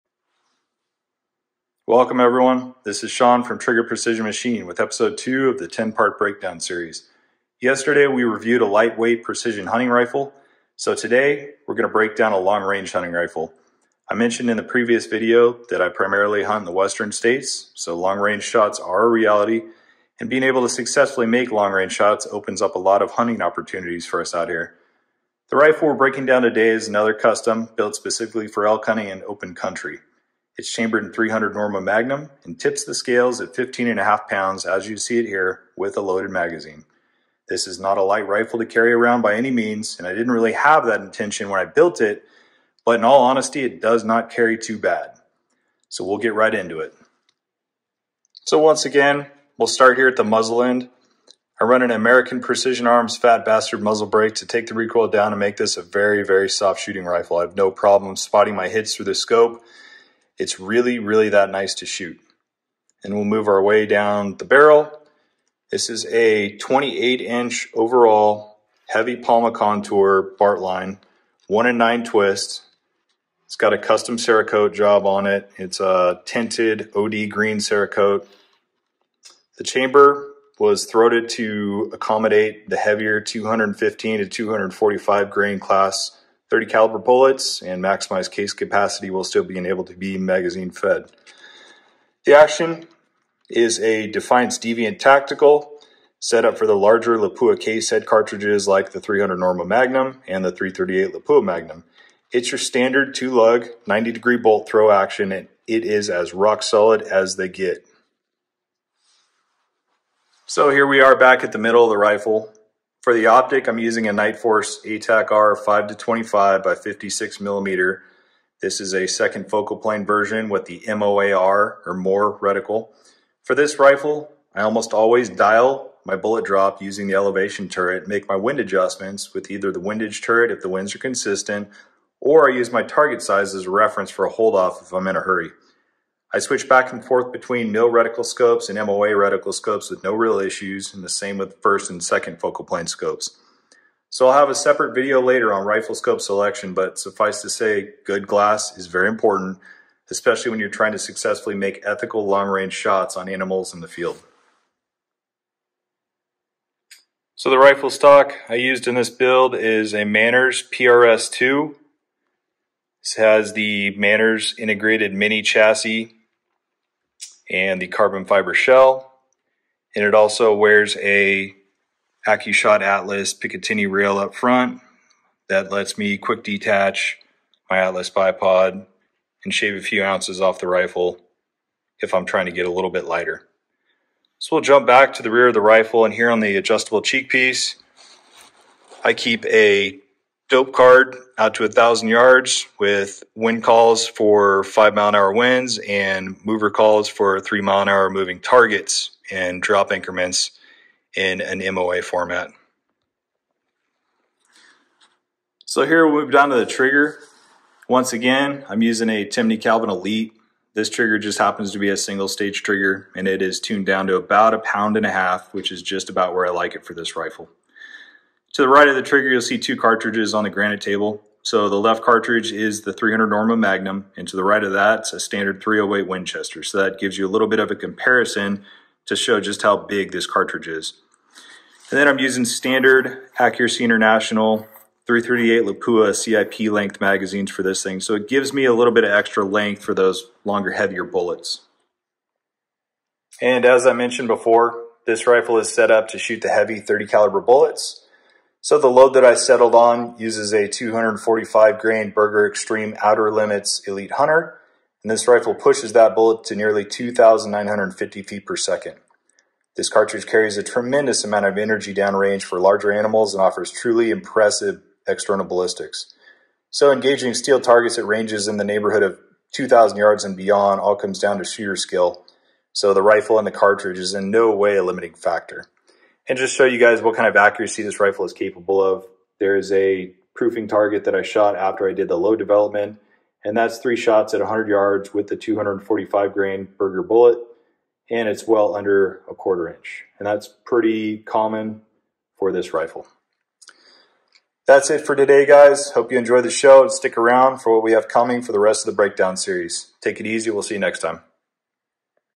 TPM Breakdown Series - #2 of 10 - 300 Norma Magnum Long Range Precision Hunting Rifle
Episode 2 of the breakdown series. This is one of my favorite rifles simply because of the accuracy it has at long ranges.

Thanks!

-TPM